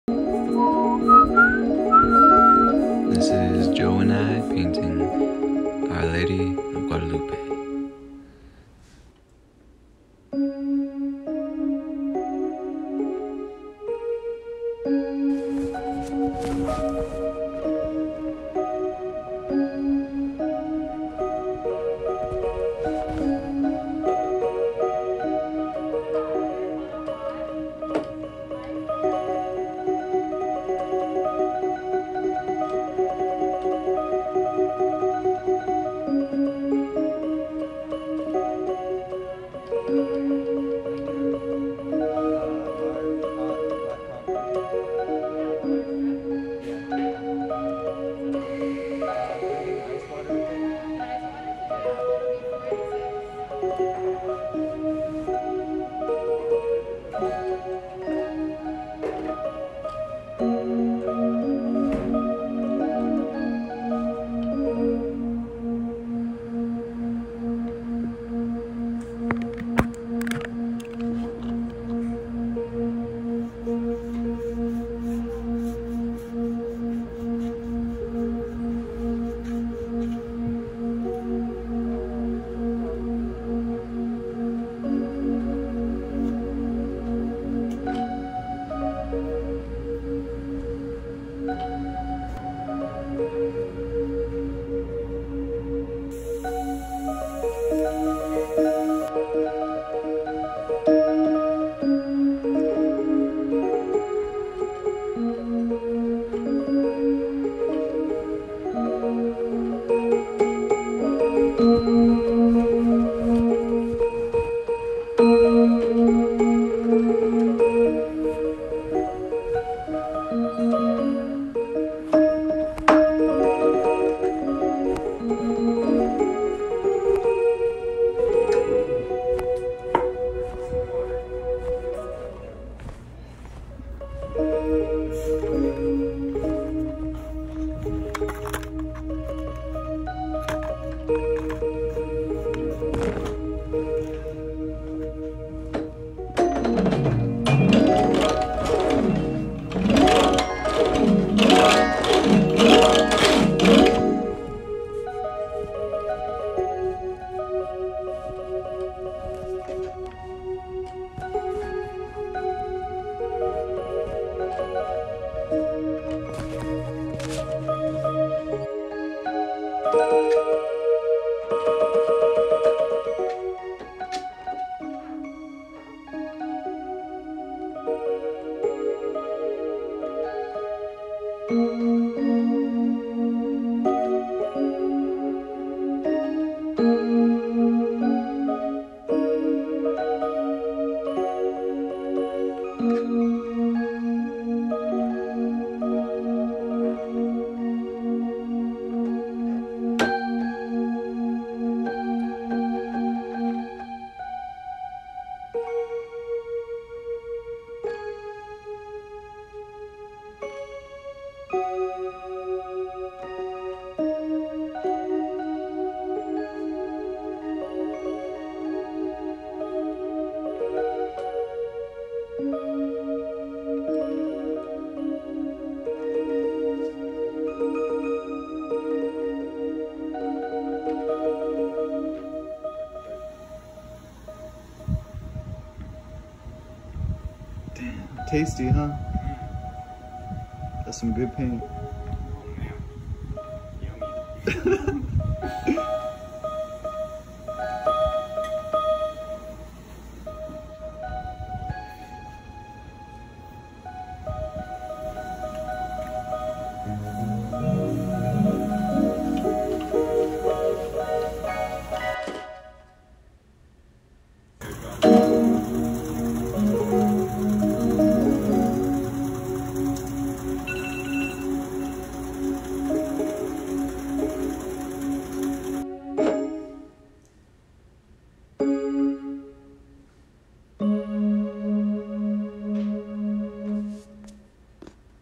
Painting Our Lady of Guadalupe
Joe and I (Bjorn)
Oil and acrylic and other mediums
Me on piano
One day paint on june 11

Peace and love and serenity